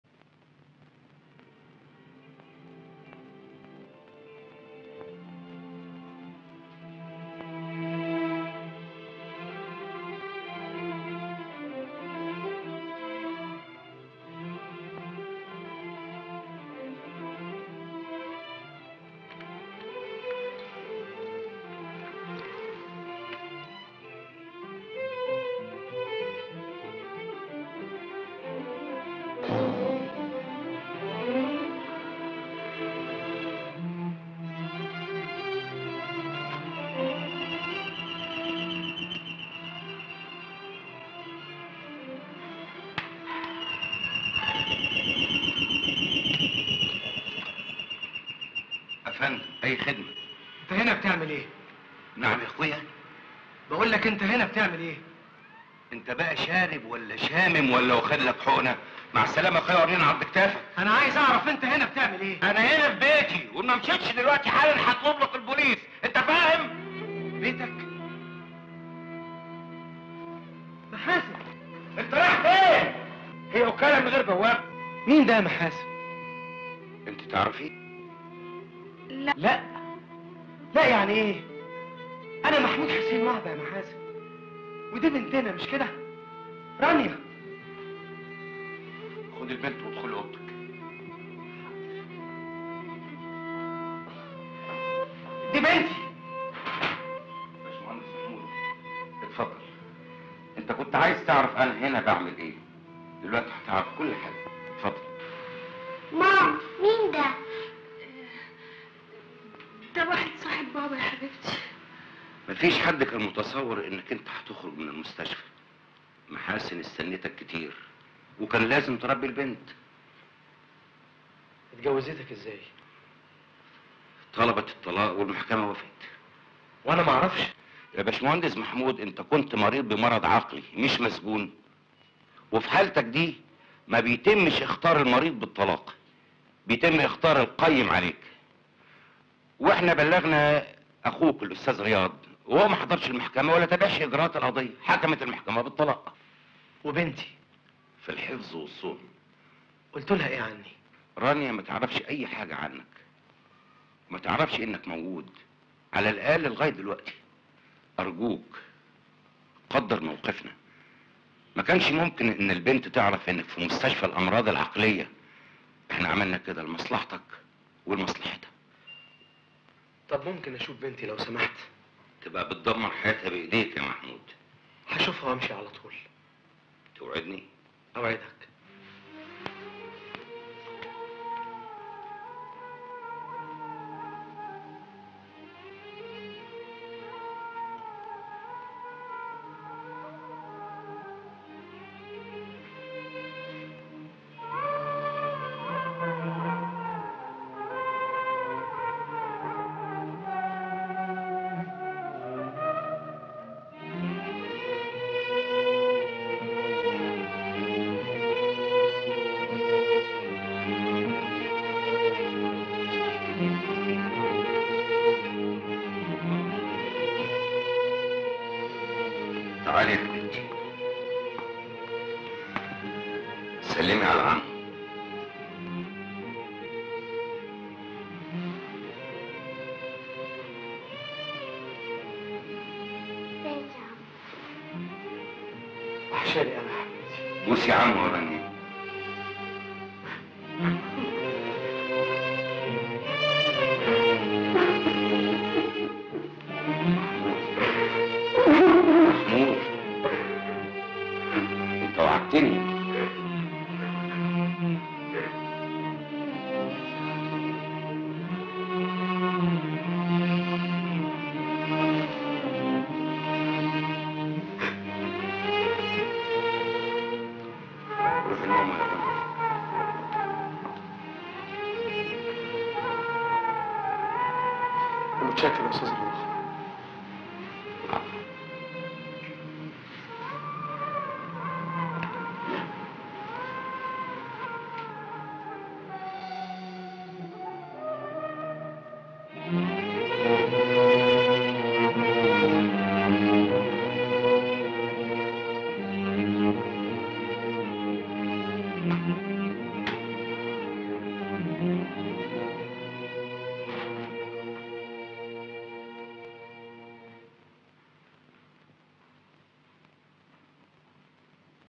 حسن يوسف خرج من المستشفى ورجع بيته اكتشف إن مراته اتجوزت
محمود مهندس ذو مبادىء يرفض الرشوة والفساد يزج به فى مستشفى الامراض النفسية ويخرج بعد سنوات ليجد زوجته طلقته وتزوجت بآخر واخوه استولى على ميراثه، فيقرر الانتقام.

بطولة:
حسن يوسف - يسرا - صلاح قابيل - هياتم وآخرون.
قصة وسيناريو وحوار: إبراهيم محمد على
إخراج: حسن يوسف